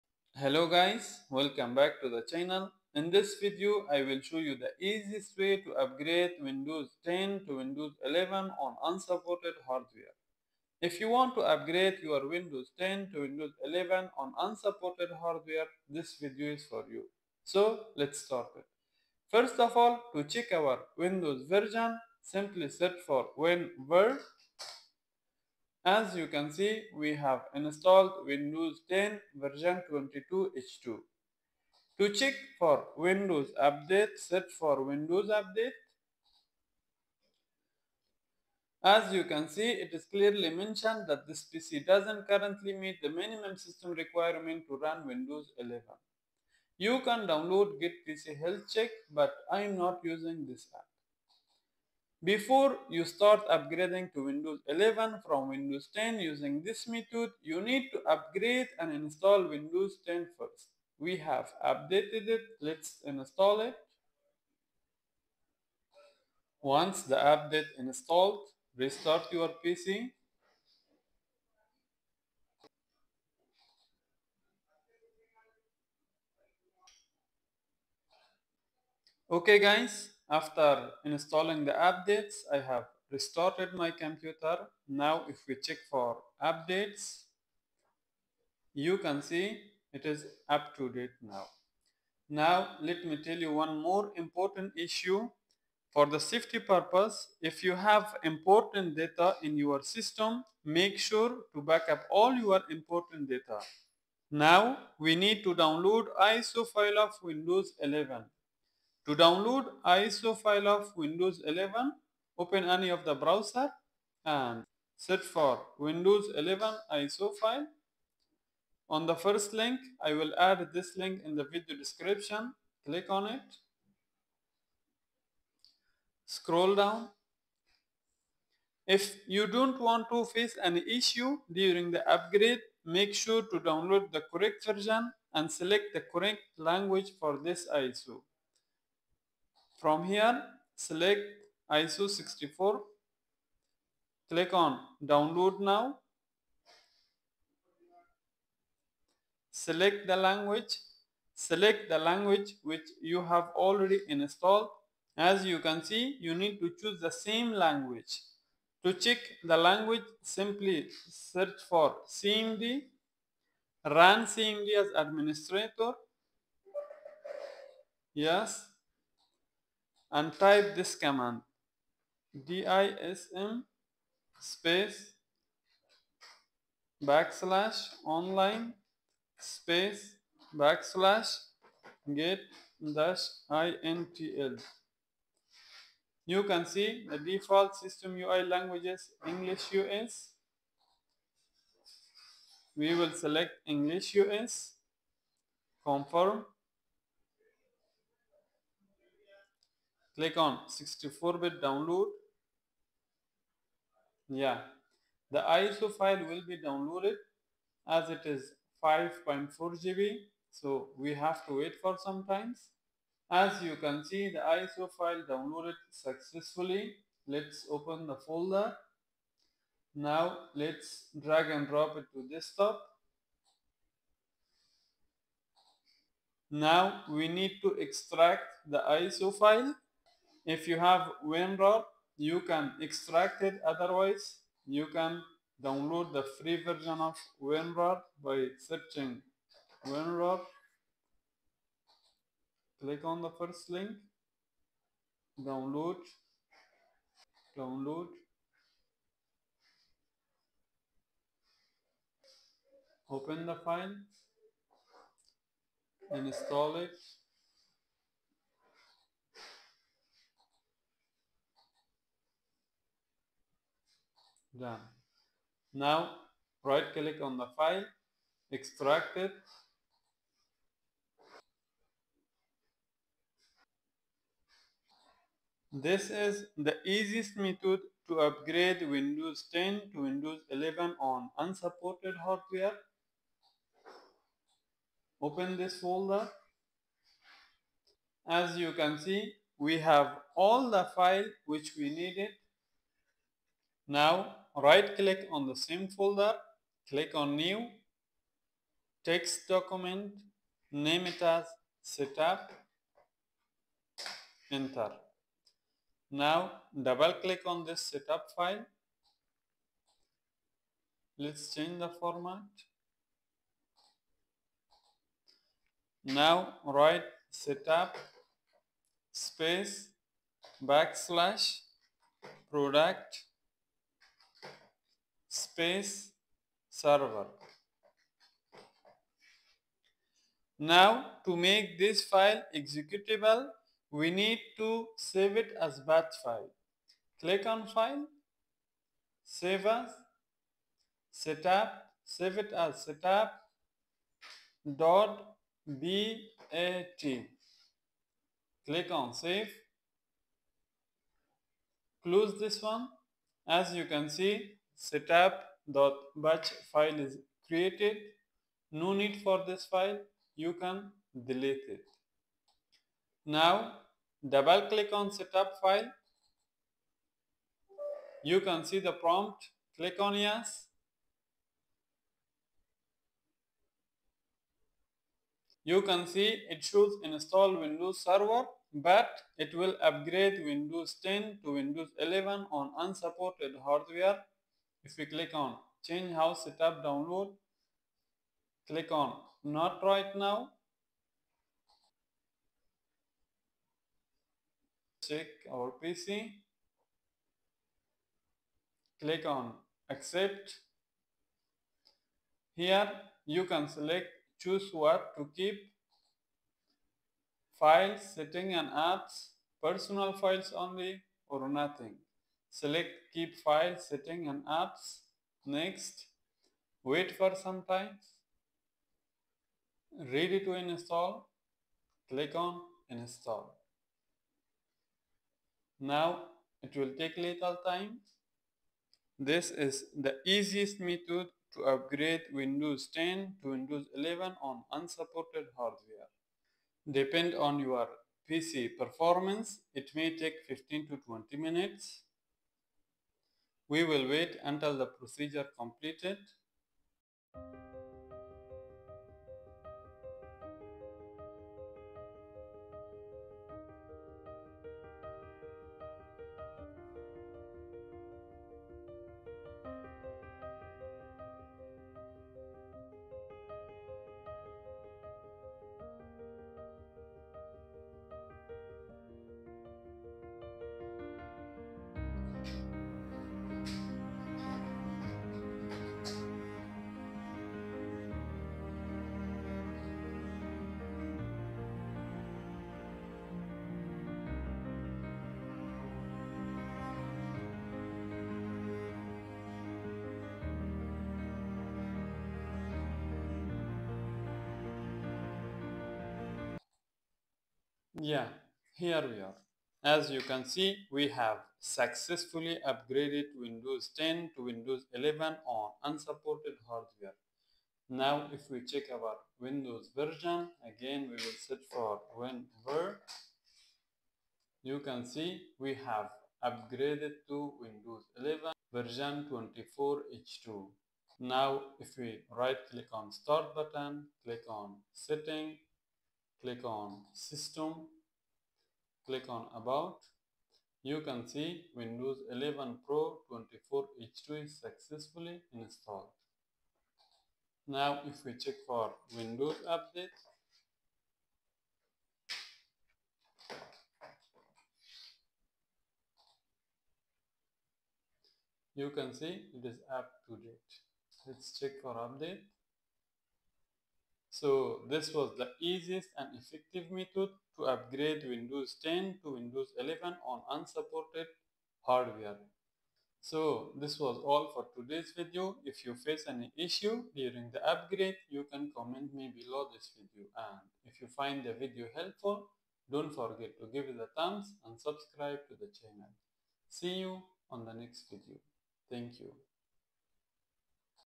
Install Windows 11 on Unsupported PC: (New) No CMD, No Data Loss
You're wondering if you can still run Windows 11 on your older PC? This video provides a comprehensive guide to navigating the challenges of installing Windows 11 on unsupported hardware, covering everything from compatibility checks to workarounds for potential issues.

🎯 This simple, straightforward method works for most older systems. I’ll take you through the process, showing you exactly what to do and how to avoid common mistakes. Let’s make upgrading to Windows 11 stress-free!
💡 What You'll Learn in This Video:
How to update windows
How to meet the minimum system requirements for Windows 11
How to prevent errors during installation
A simple and easy method to install Windows 11 without CMD
How to troubleshoot errors like installation hangs or failure
Does upgrading from Windows 10 to Windows 11 cost anything?

⚠️Important Note:
This method follows Microsoft’s official process but includes a bypass for TPM 2.0 and Secure Boot requirements, making it compatible with unsupported systems.